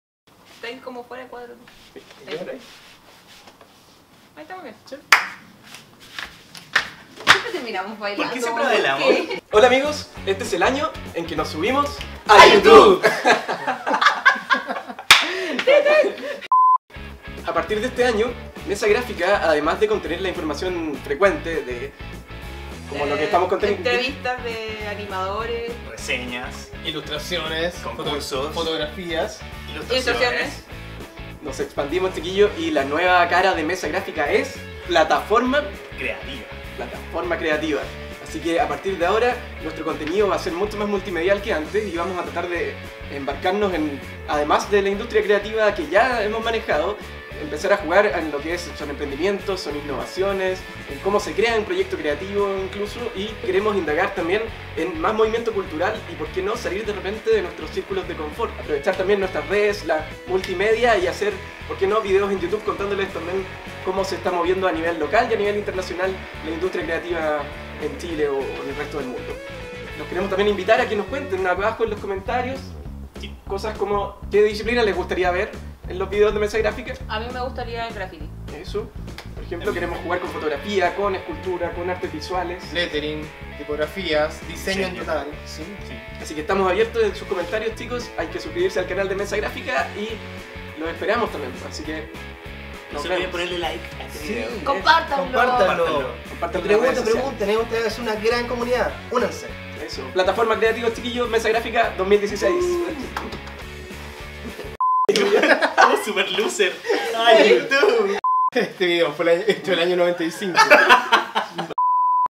MesaGráfica.cl expande su línea editorial | Mesa Gráfica
Así es. Pero tranquilos, no dejaremos de lado la ilustración, la narrativa gráfica ni la animación. Ponle play al video, entérate de nuestros planes y ayúdanos con tu comentario.

Locación y agradecimientos a: Taller Holanda.